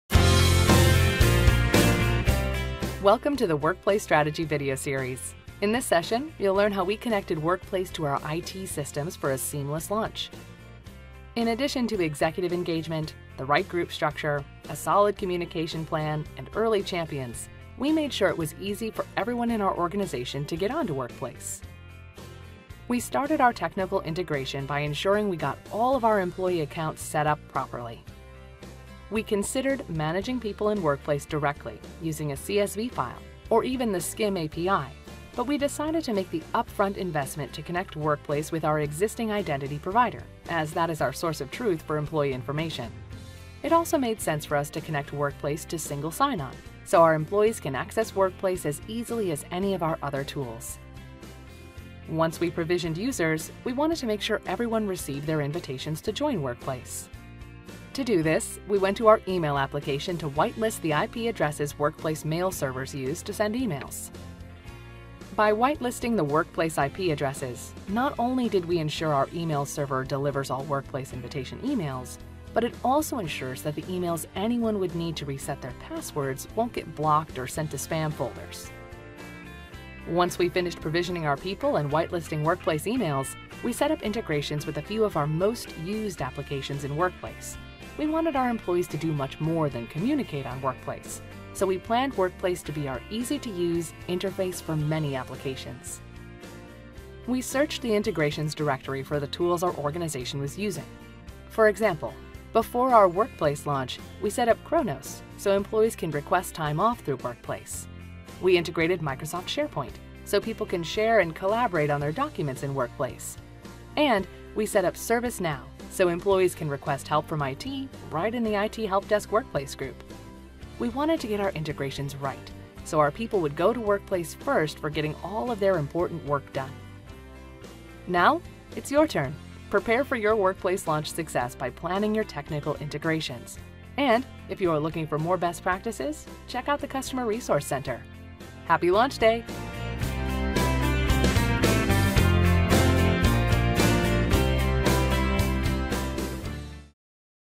Technical integration: Connect Workplace to your IT systems
Workplace Partner, Generation Digital can facilitate a workshop with your IT and Security teams to develop a strategy to Provision & Authenticate users and Connect Workplace to your IT systems.

Integrating Workplace into your IT systems sets your organisation up for long-term success and security. The goal is to make it as seamless as possible to manage accounts as people join and leave the organisation. Additionally, we want to ensure that people have the best experience with Workplace, regardless of whether they are working from a mobile device or desktop